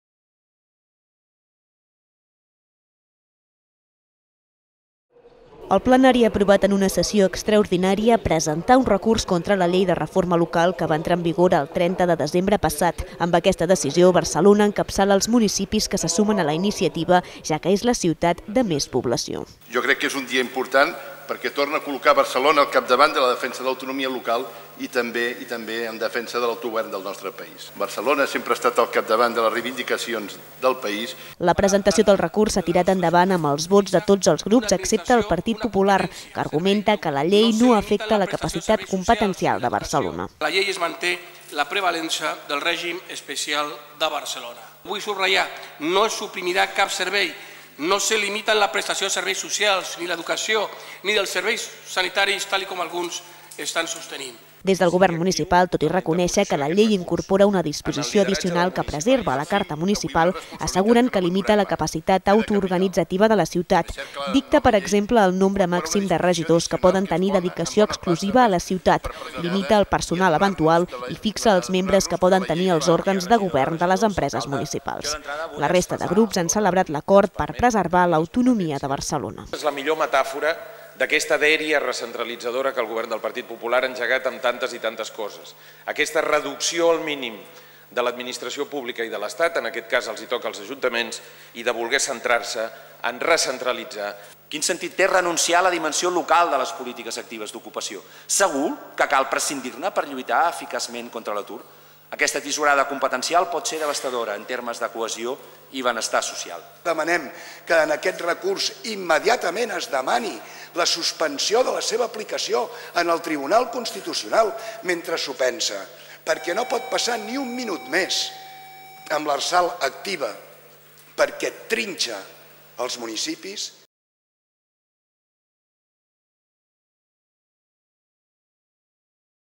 Barcelona recorrerà la llei de reforma local al Tribunal Constitucional
El Plenari del Consell Municipal ha aprovat  amb els vots de CiU, PSC, ICV-EUiA i UpB, la presentació d'un recurs davant el Tribunal Constitucional contra la Llei de Racionalització i Sostenibilitat de l'Administració Local, que està vigent des del 30 de desembre de 2013. El Plenari ha donat llum verd a iniciar els tràmits en entendre que la llei vulnera l'autonomia local.